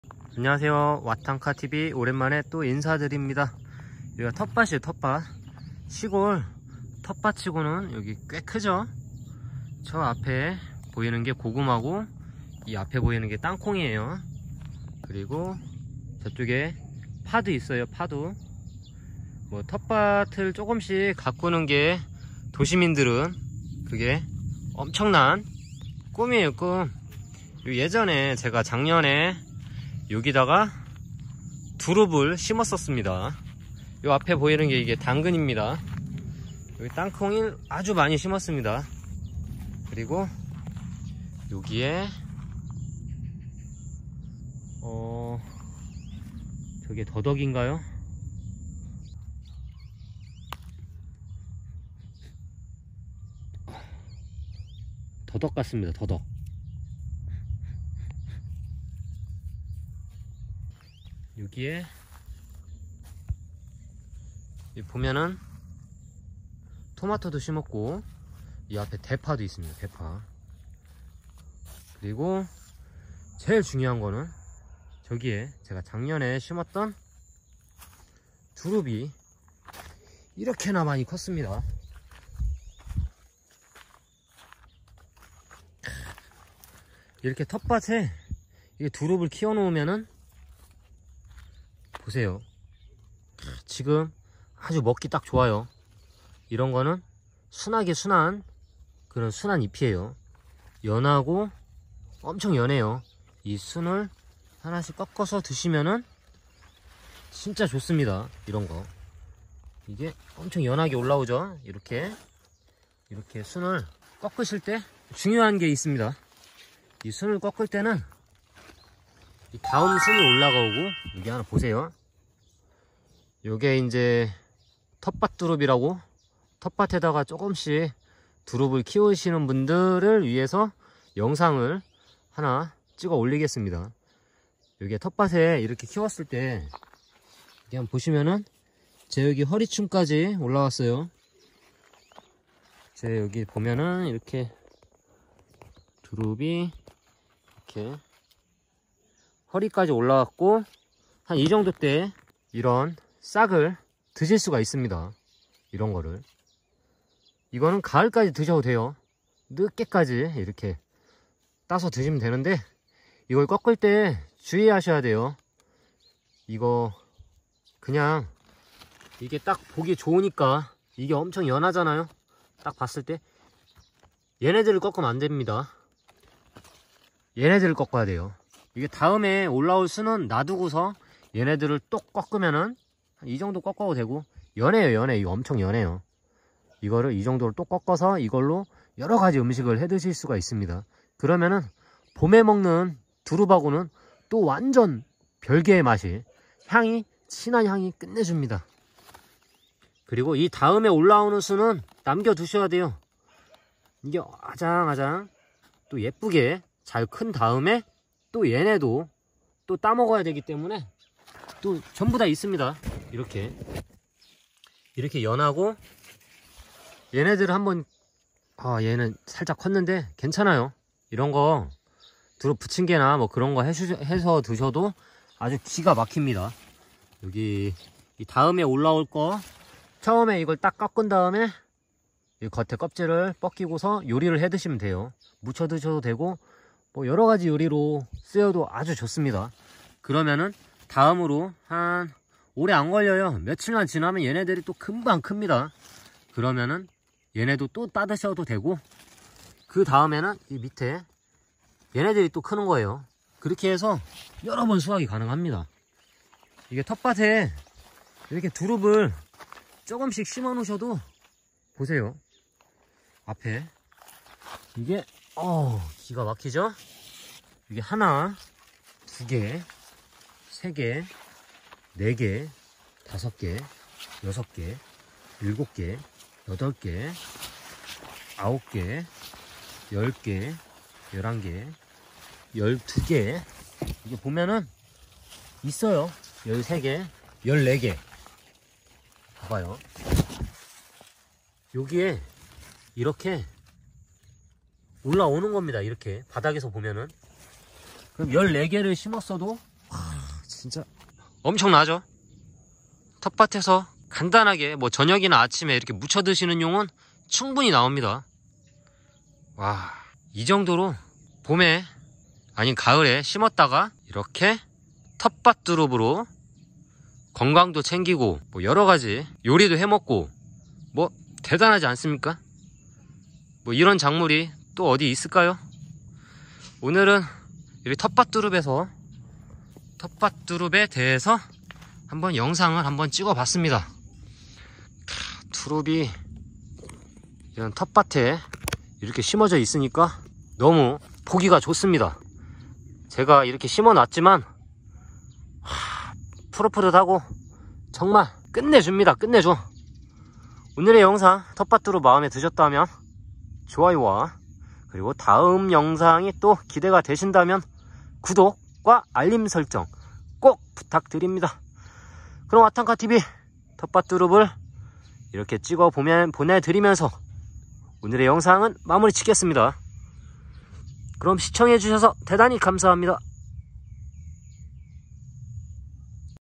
땅두릅순 채취요령 알려드려요./가을까지 땅두릅먹기/텃밭두릅전성시대/
땅두릅 순꺽는방법 알려드립니다.